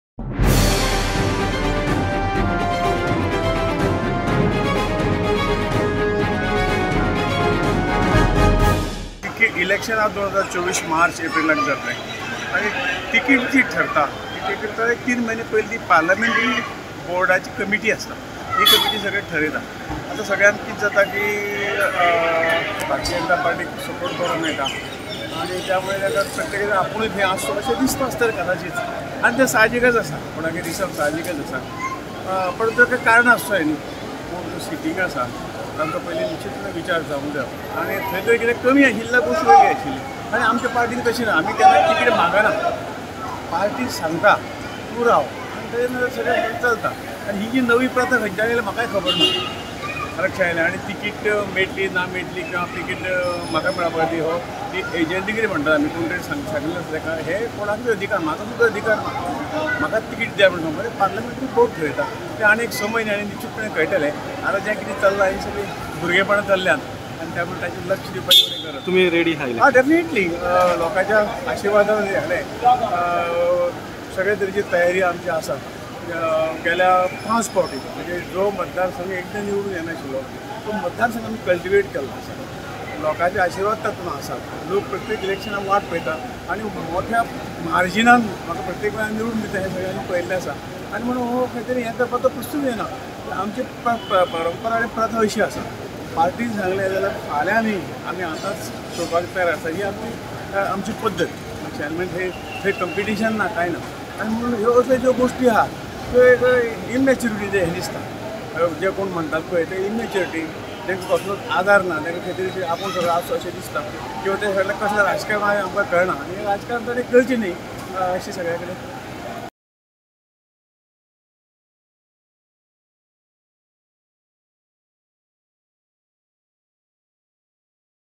SHRIPAD READY FOR ANOTHER GO
The MP ticket is decided by the parliamentary board. People who speak on the issuance of MP tickets are immature and stand no grounds.